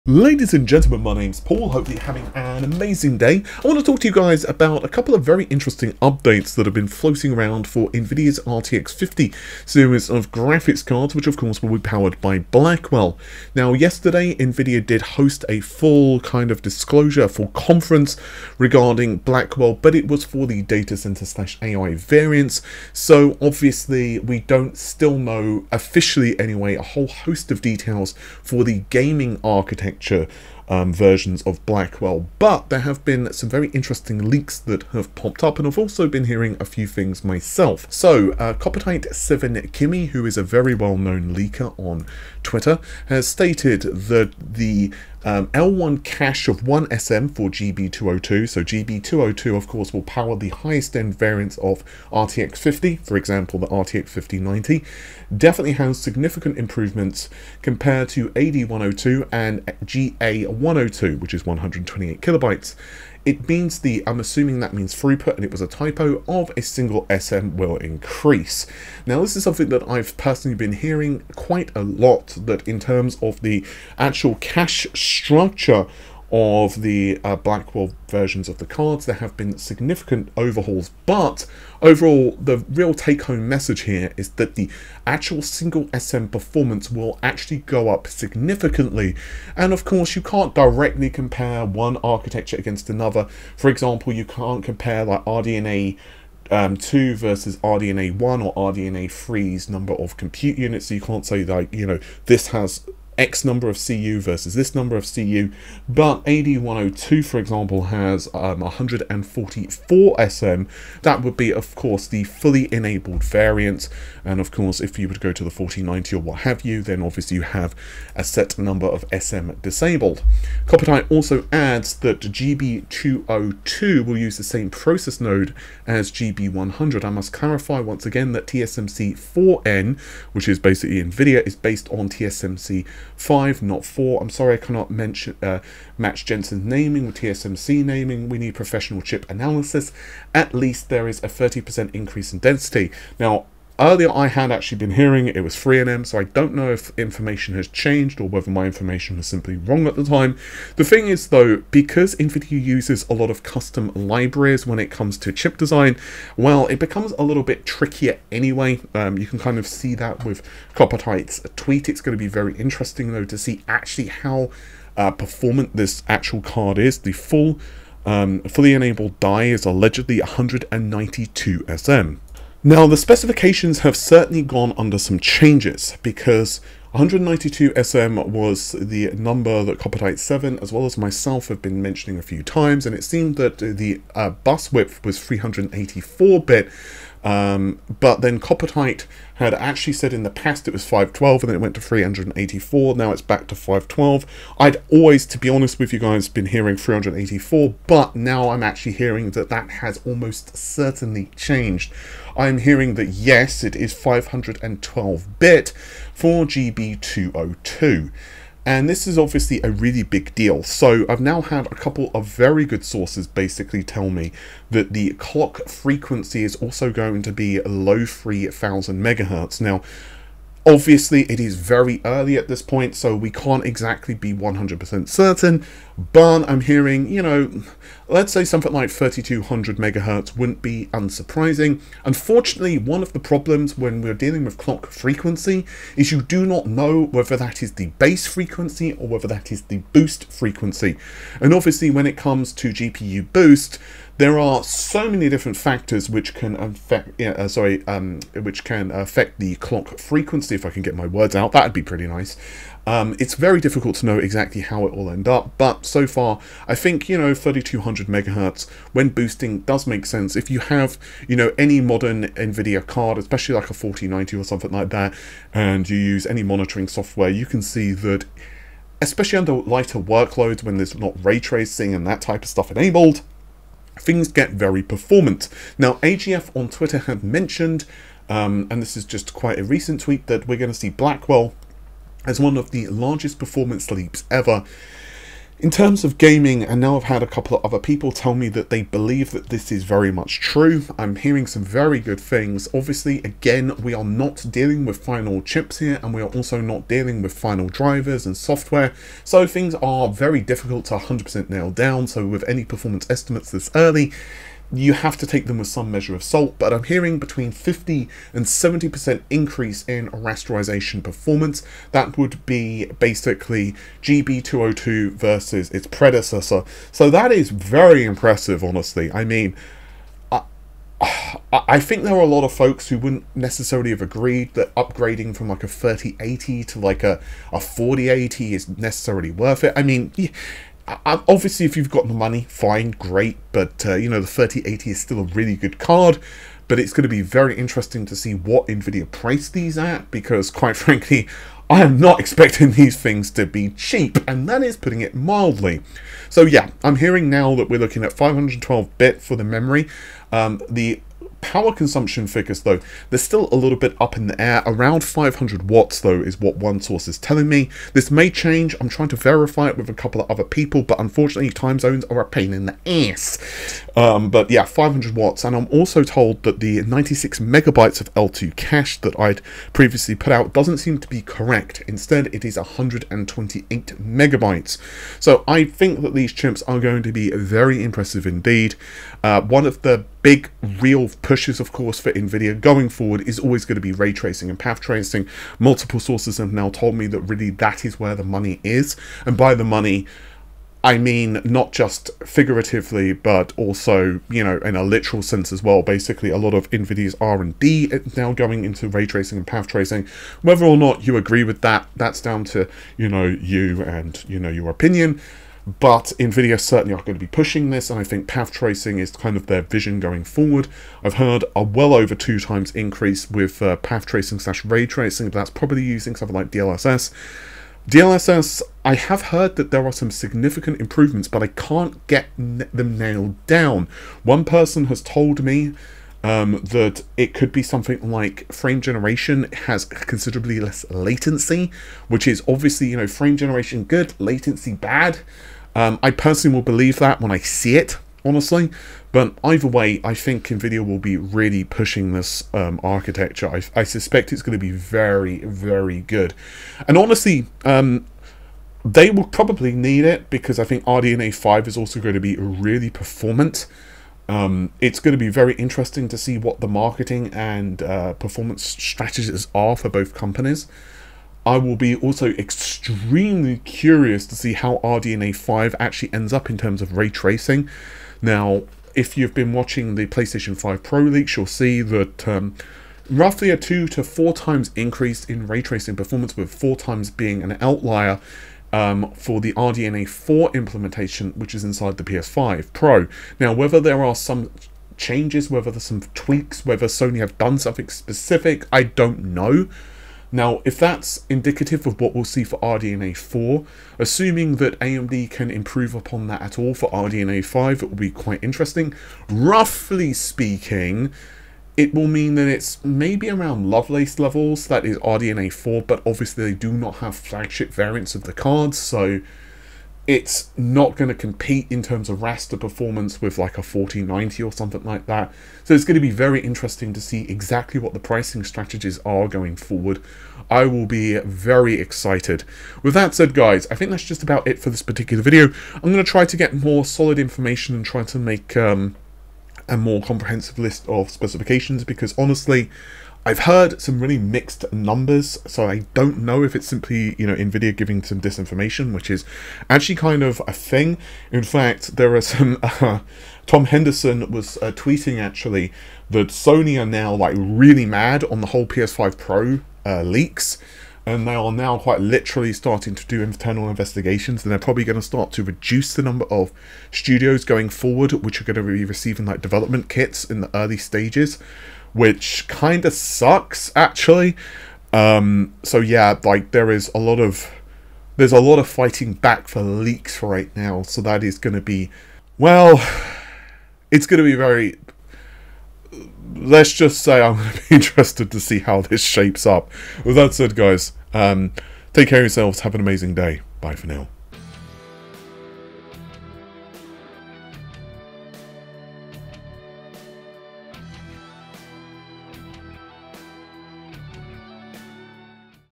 Nvidia's SECRET Weapon | RTX 50 HUGE SM & Cache Changes
Hot on the heels of Nvidia unveiling the Blackwell AI GPU, some fresh leaks for RTX 50 Blackwell gaming GPU have leaked. These latest leks on the RTX 50 specs and architecture unveil some HUGE changes to the SMs and cache of Nvidia's RTX Blackwell, giving us more insight into the next gen Nvdia GPU.

Join us as we examine the latest leaked info on RTX 50, which again includes updated hardware specifications, gaming and ray tracing and path tracing performance, and some very interesting insights into the RTX 50 architecture improvements. But what about RDNA 5? Will AMD be able to compete with this MONSTER graphics card?